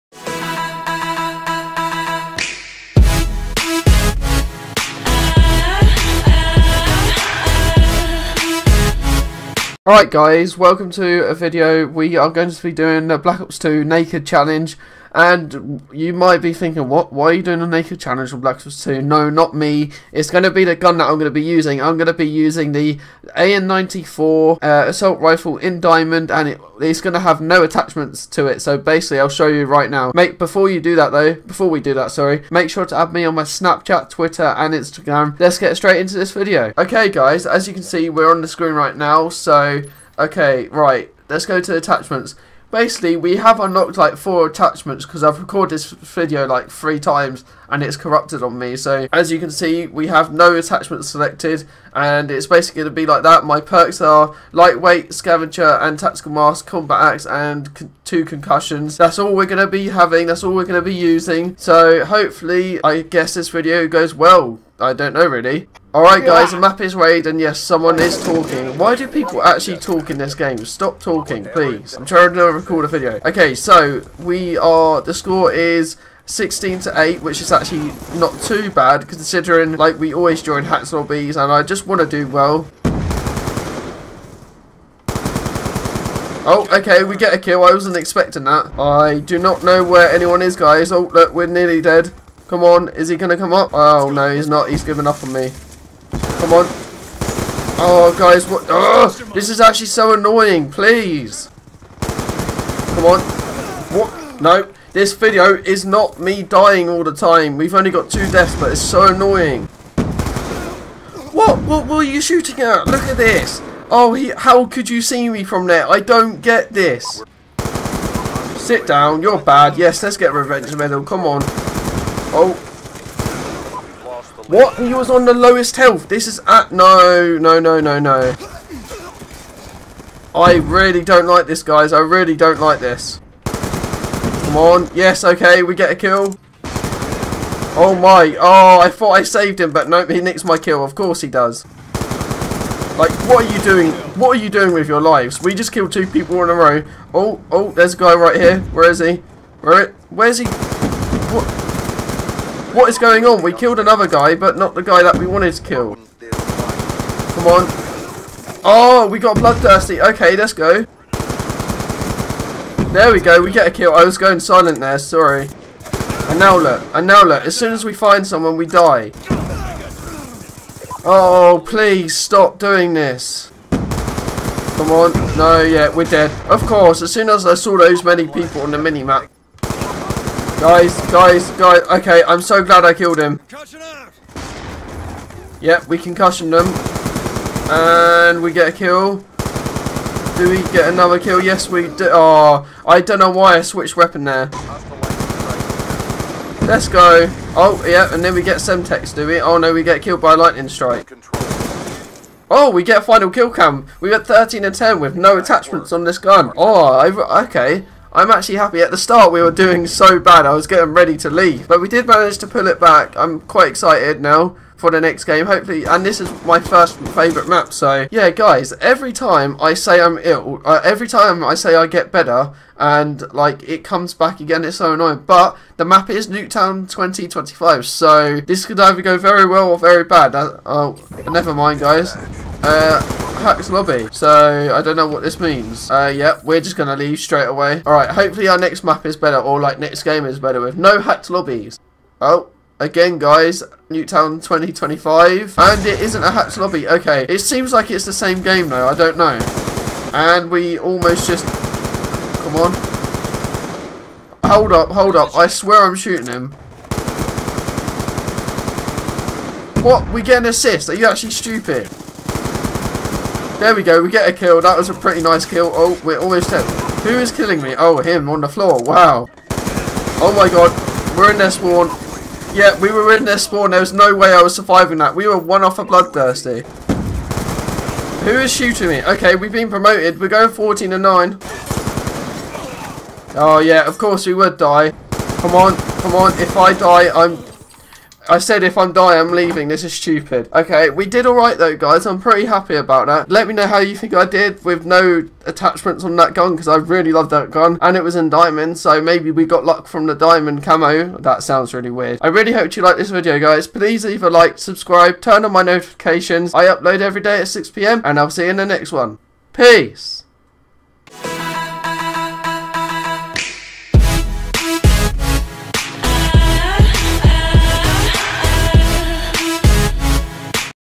THE NAKED GUN CHALLENGE ON THE AN-94 ASSAULT RIFLE!!!
Hey guys In this video I do the naked gun challenge, It means that my gun that I play In this video, has no attachments on It. I thought I would do this because I wanted to see what It Is like to have no attachments on that gun, As that Is one of my favourite guns!!!

► Instagram izacdodds

Hoped you liked the video :).